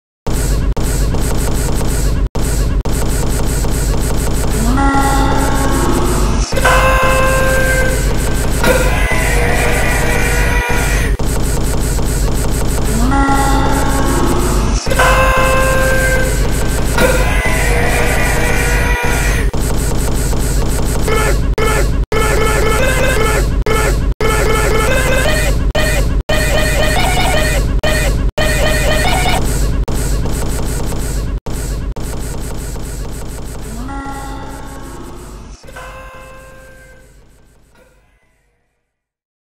I SPENT 100 DAYS ON A MILITARY AIRSHIP [YTPMV]
Something silly I made like, a year or so ago. I assumed it was lost forever but I found it on my Google Drive today.